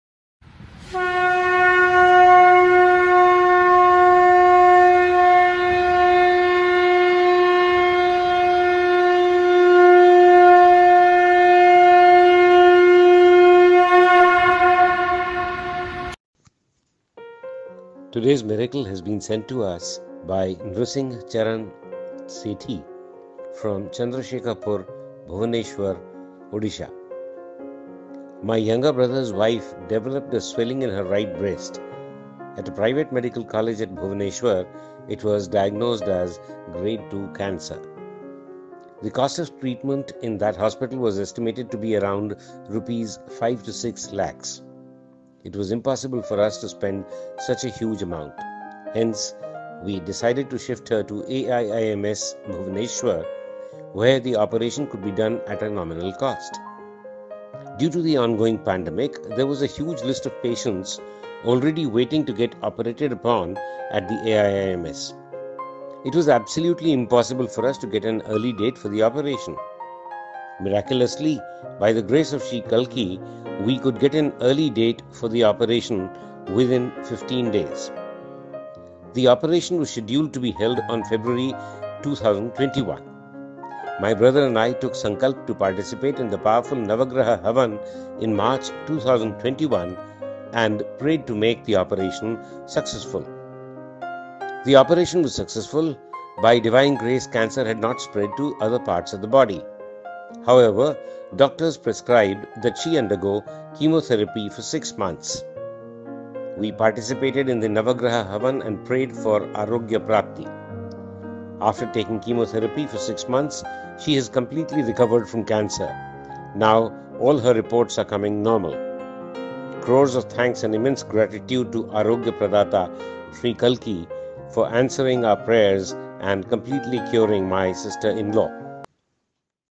At every step of recovery from cancer, Sri Kalki’s grace was there with my sister-in-law.
Sri Kalki Miracles